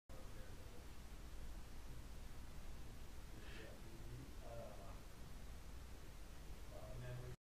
Sshh Sallie
the static full spectrum 1080p camcorder positioned in the upstairs Master Bedroom of the Sallie House in Atchison, KS picked up a sshhing sound as though those investigators upstairs were making too much noise.